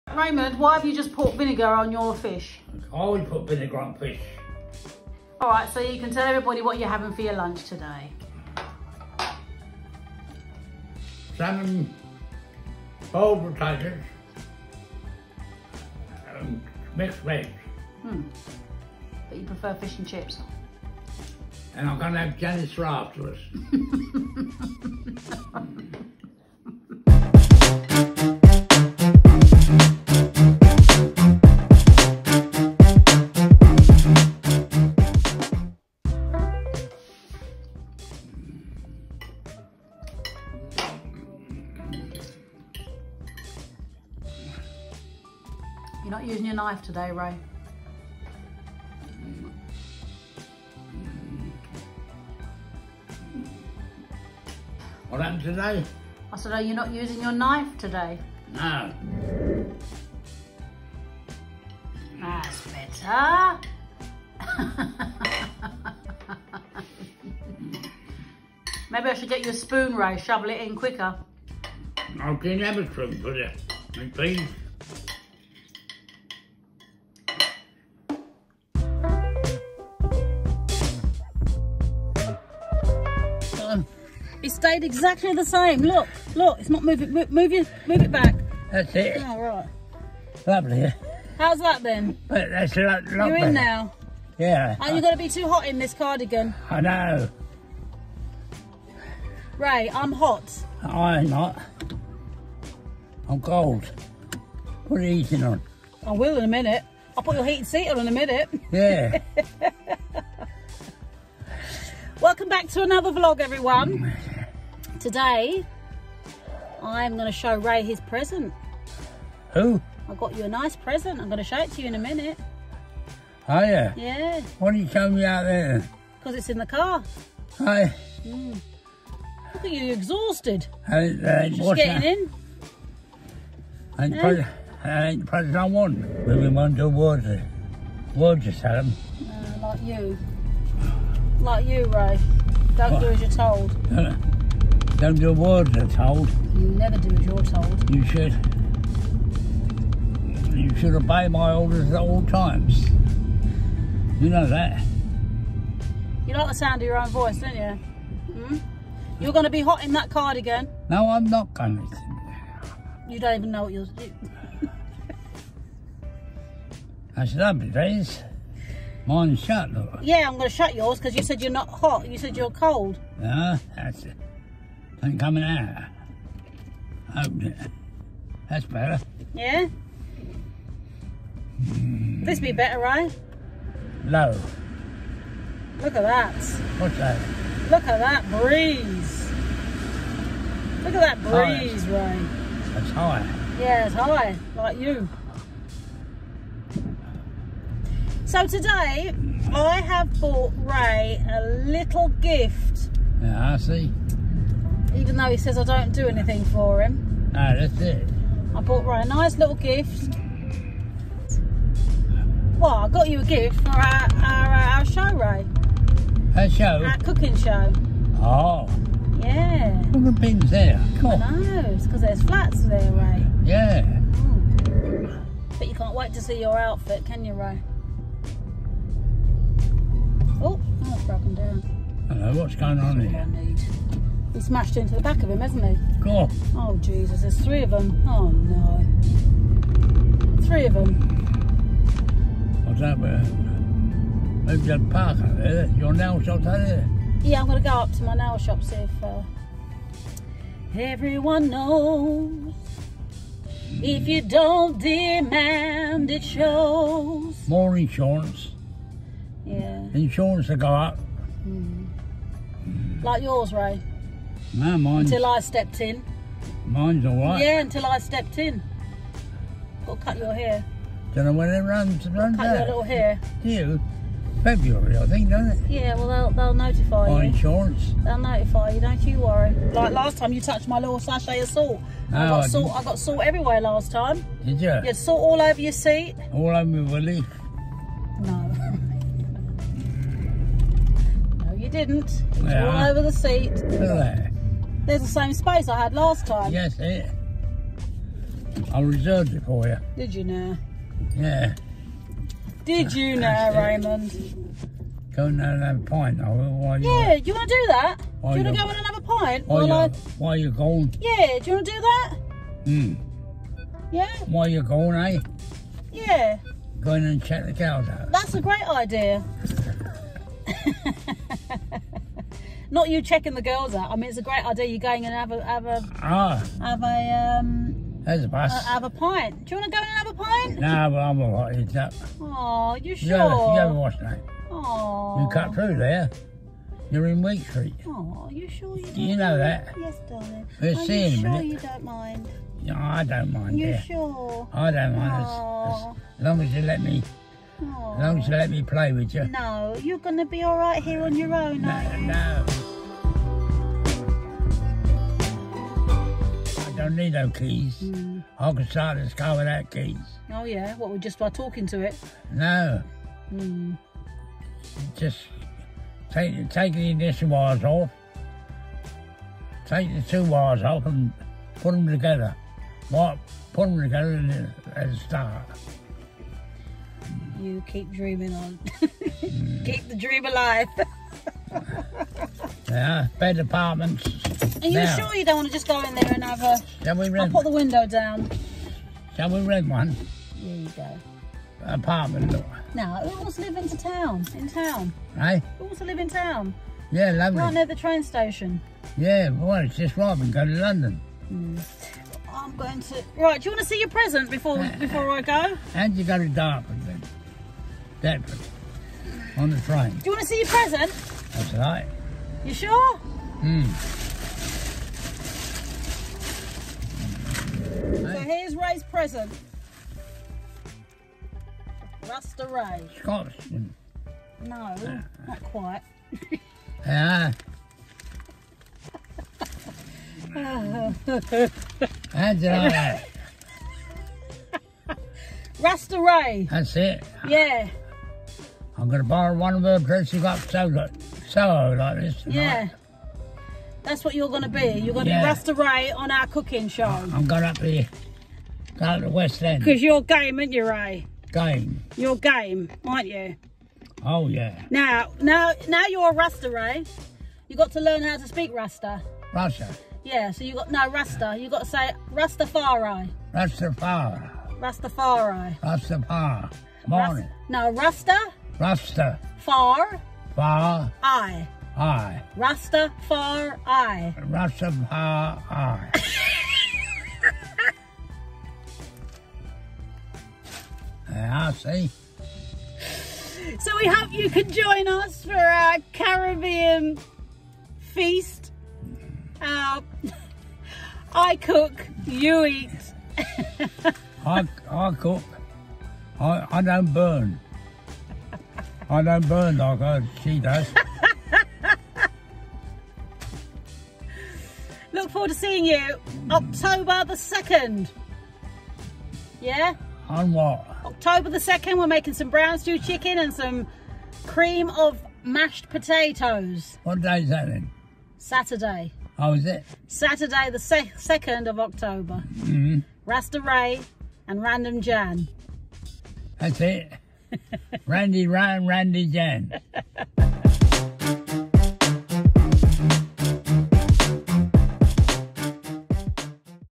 Giving a surprise Gift 🎁
So Here is another Vlog Ray absolutely loves to dress up and he loves to raise a laugh. This vlog our day out and me showing him his gift for a live cooking show starting on 2 October on Instagram live I have another present for Ray that you will see on the day look forward to seeing you all 6 PM Instagram live.

Thank you for your love and support for this channel❤️